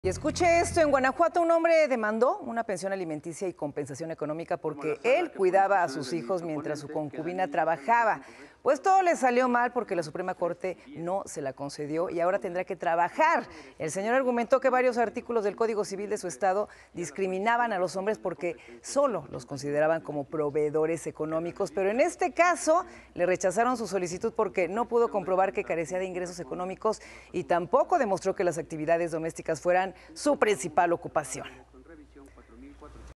Le niegan pensión alimenticia a un hombre que cuidaba a sus hijos | Noticias con Francisco Zea
Le niegan pensión alimenticia a un hombre que cuidaba a sus hijos en Guanajuato.